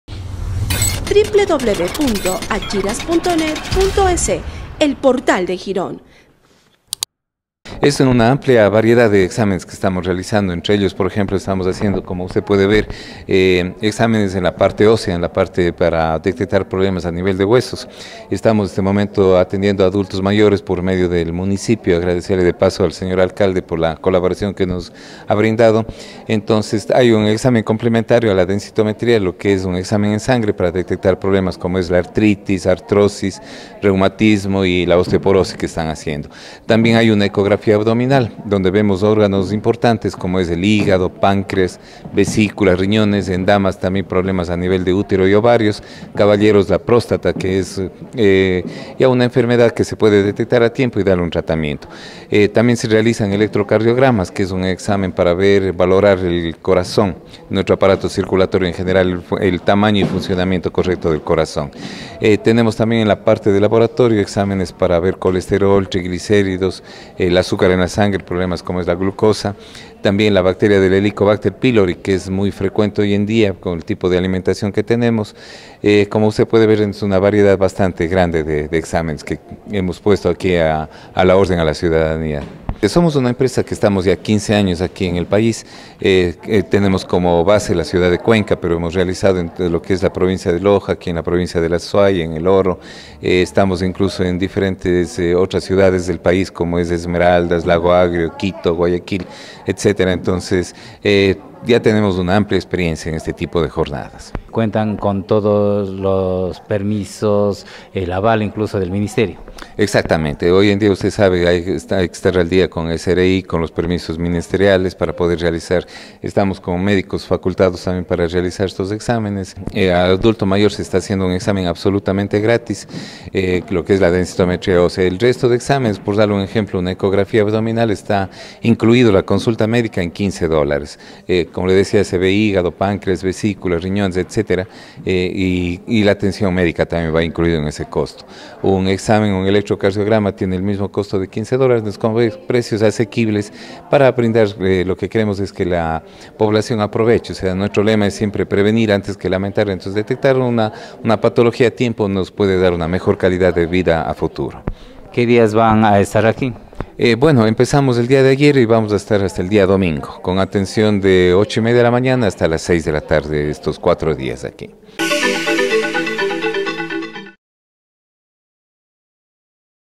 Jornada Médica se cumple en Salón del Pueblo hasta el domingo
Desde el 23 al 27 de agosto la empresa DISA Cia. Ltda., realizará en Girón diversos exámenes médicos a bajos costos, manifestó Pablo Abad director de la empresa, quien acotó tienen 15 años de experiencia.
La jornada se ha coordinado con el GAD Municipal de Girón y este viernes se hicieron exámenes gratuitos de densitometría ósea a adultos mayores que asisten a la Casa del Adulto Mayor.
Los exámenes que se practican son: en sangre de artrosis, artritis y fiebre reumática; osteoporosis, prevención del cáncer de próstata, examen para detectar la bacteria helicobacter pylori, electrocardiogramas.